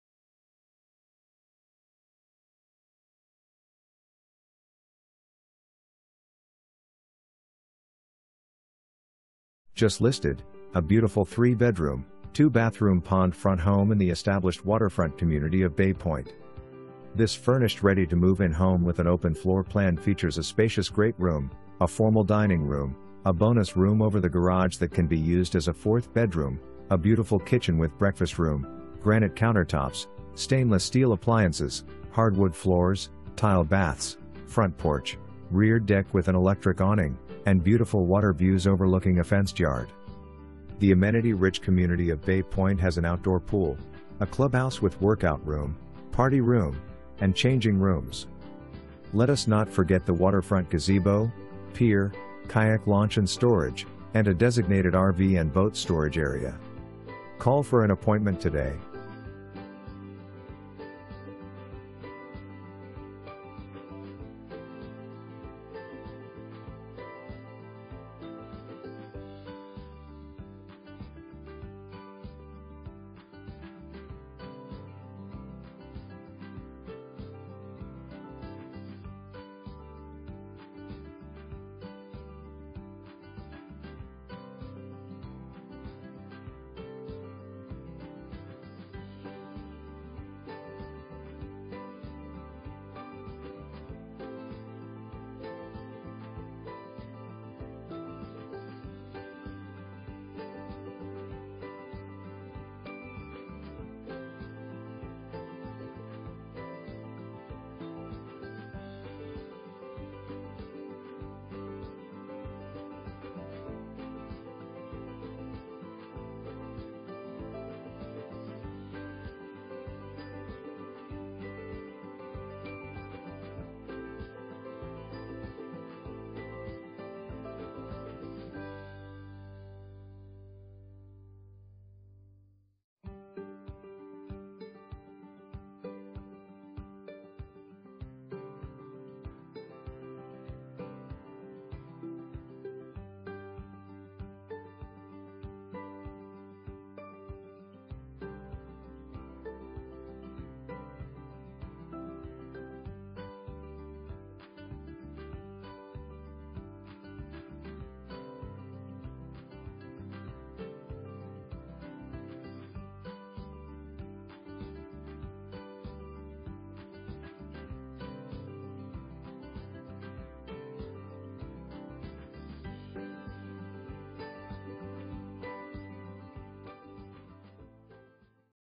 Homes for Sale - 34008 INLET BREEZE DRIVE, LEWES, DE
34008 INLET BREEZE DRIVE
LEWES, DE 19958
$478,000, 3 bed, 2.00 bath, 2,028 SF, MLS# DESU179560

Just listed, a beautiful 3-bedroom, 2-bathroom pond-front home in the established waterfront community of Bay Pointe. This furnished ready to move-in home with an open floor plan features a spacious great room, a formal dining room, a bonus room over the garage that can be used as a 4th bedroom, a beautiful kitchen with breakfast room, granite countertops, stainless steel appliances, hardwood floors, tile baths, front porch, rear deck with an electric awning, and beautiful water views overlooking a fenced yard. The amenity-rich community of Bay Pointe has an outdoor pool, a clubhouse with workout room, party room, and changing rooms. Let us not forget the waterfront gazebo, pier, kayak launch and storage, and a designated RV and boat storage area. Call for an appointment today.

2 full bath

Bill Cullin
Long & Foster Real Estate, Inc